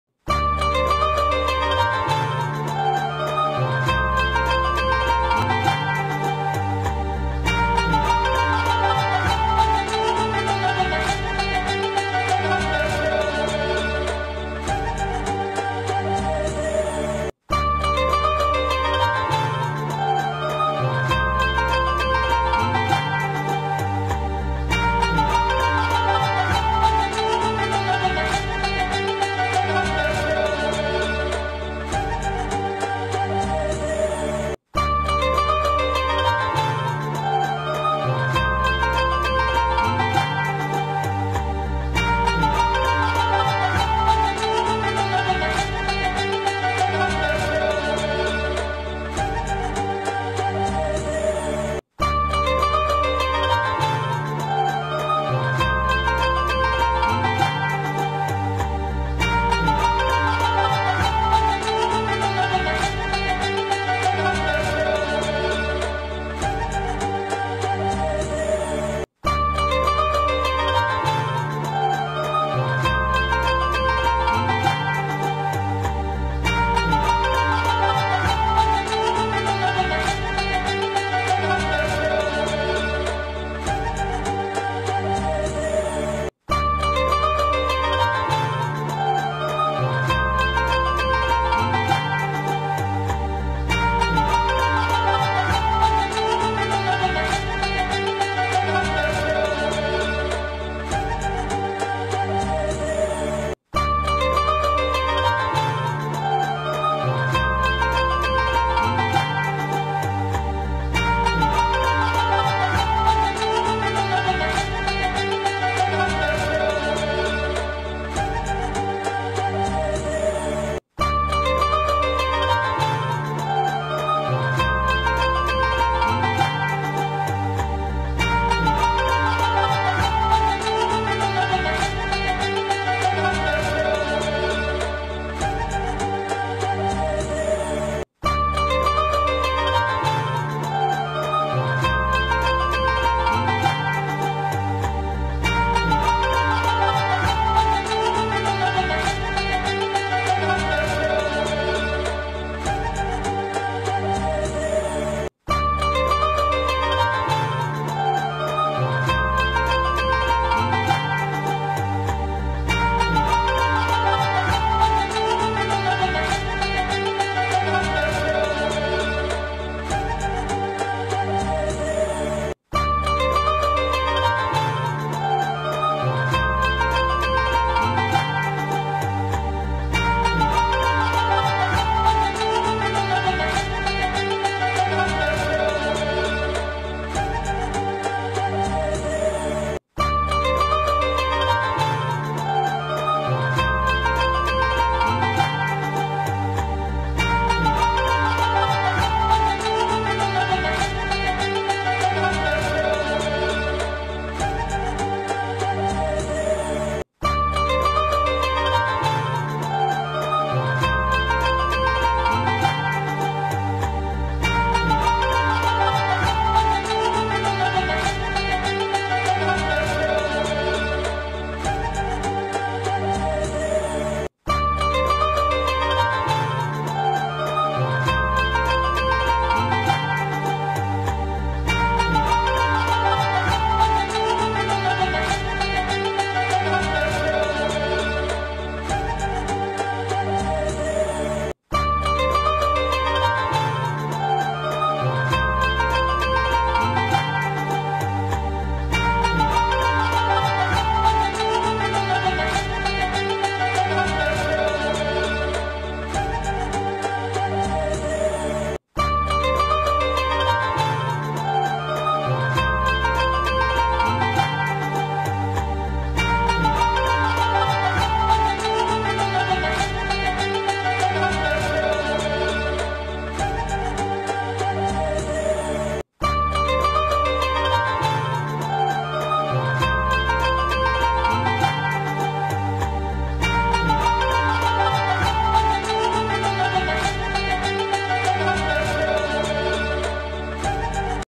تحلية أو ياغورت بالفراولة Dessert aux fraises
طريقة سهلة وسريعة لعمل تحلية أو ياغورت الفرولة بقوام كريمي ومذاق رائع 👌